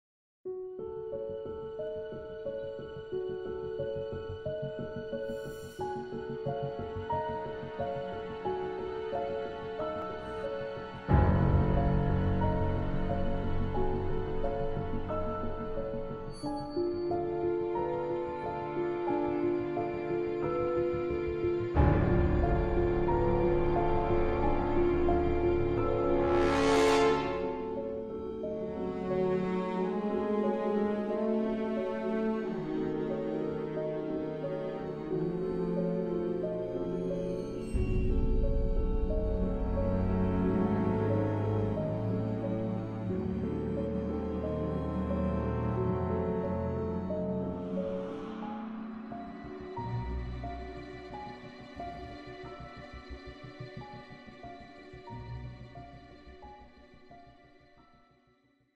Awesome Lightning in Slow Motion
Slow motion clips of lightning after a thunderstorm I recorded in my front yard